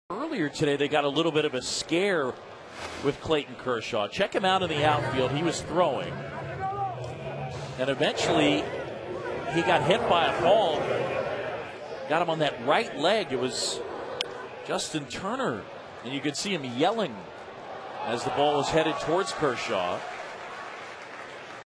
Fenway Bro Has Been There Before
A baseball that makes its way into the stands is usually a prized possession that elicits celebration for being won. Usually.